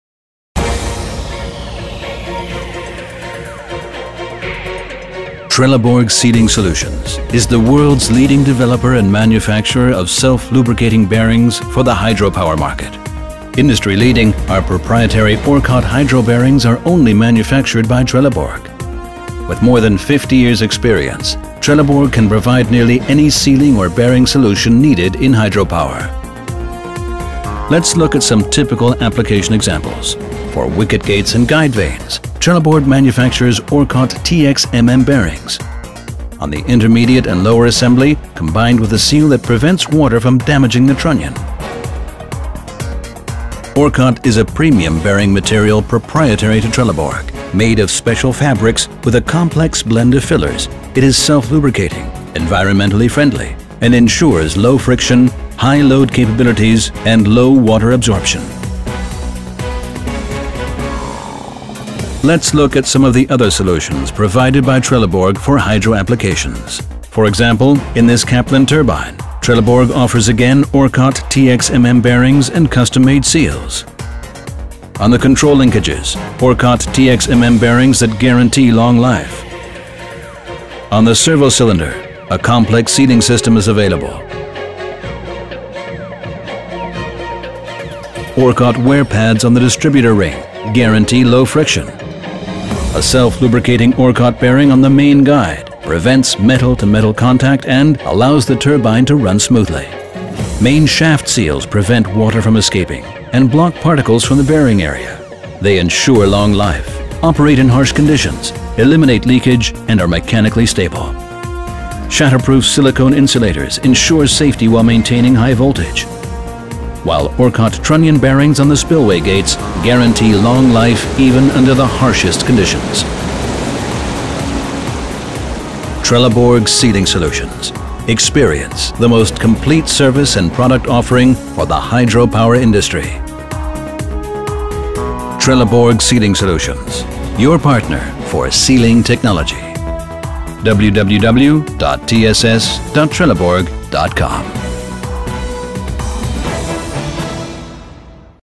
Orkot® Hydro Bearings: Hydropower Sealing & Bearing Solutions — Trelleborg Sealing Solutions
Orkot® Hydro Bearings for the hydropower industry.

Orkot® is a composite material consisting of technical fabrics impregnated with thermosetting resins, evenly dispersed solid lubricants and other additives. Orkot® bearings offer significant advantages over traditional metal bearings: they are dimensionally stable, have excellent wear resistance and outstanding low-friction characteristics, giving them unrivalled performance in dry running conditions or with boundary lubrication. With virtually no swell in seawater, they are ideal for marine and hydropower applications.

Chapters
00:00 Introduction
00:09 Sealing & Bearing Solutions for Hydropower
00:29 Orkot® TXMM Bearing
00:42 Orkot® material
01:02 Sealing solutions for hydropower applications
02:05 Products & Services for hydropower industry

Are you looking for composite bearings and thermoplastic bearings for a wide range of applications?